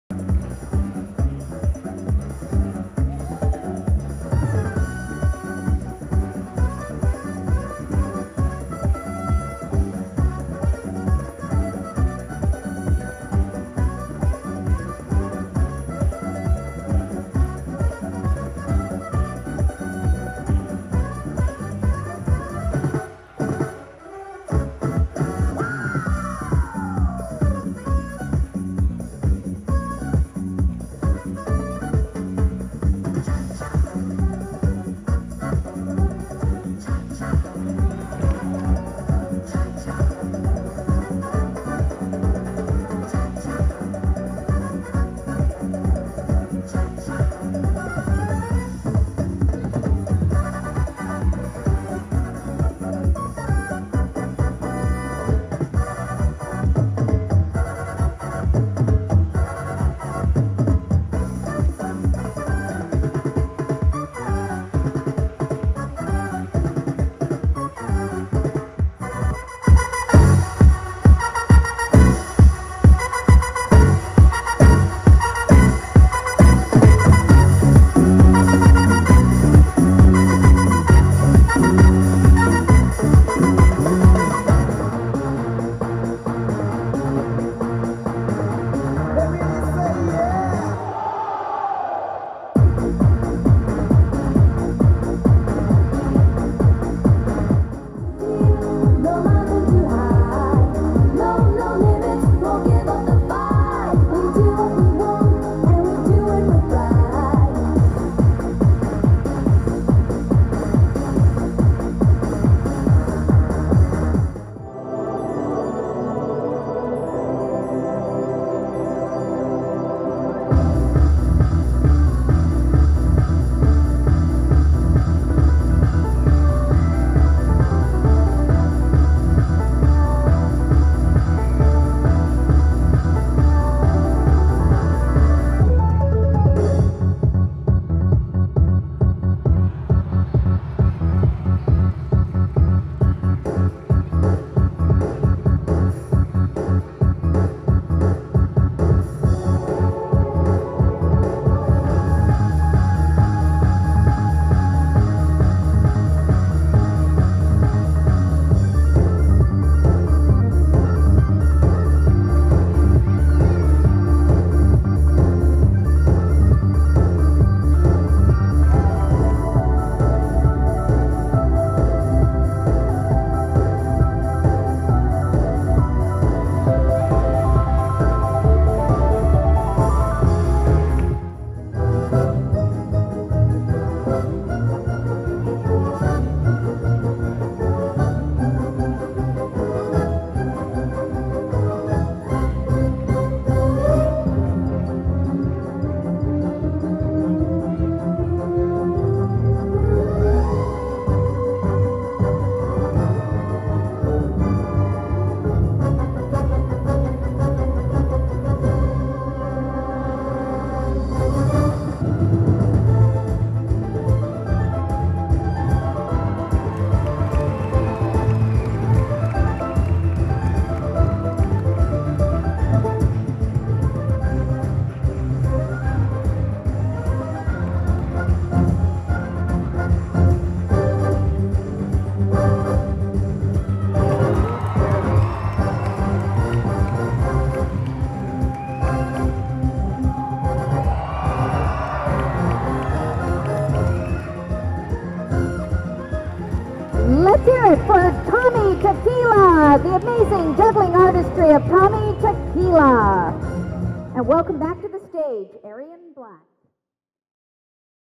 Tommy Tequila ll World's Fastest Juggler.
This is Tommy Tequila.  I happened upon him when visiting a friend of mine who was traveling with Ariann Black's show, Alakazam.

I hear this guy is one of the world's best [and fastest] jugglers.